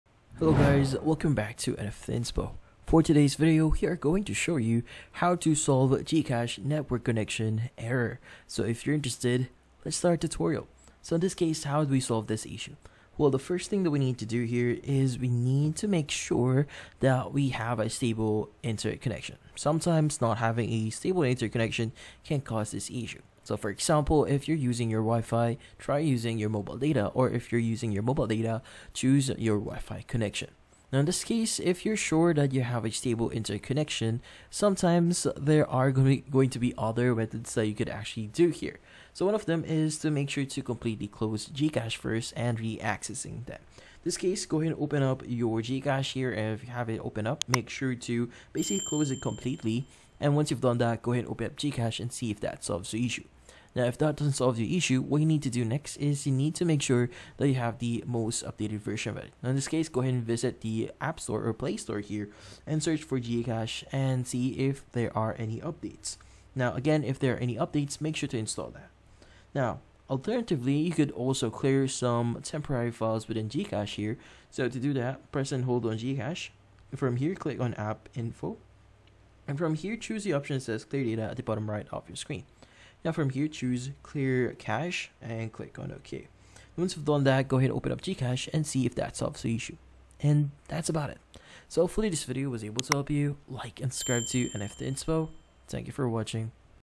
How To Solve GCash Network Connection Error (2025)
If you want to be able to solve the network connection error in GCash, then this video will be perfect for you!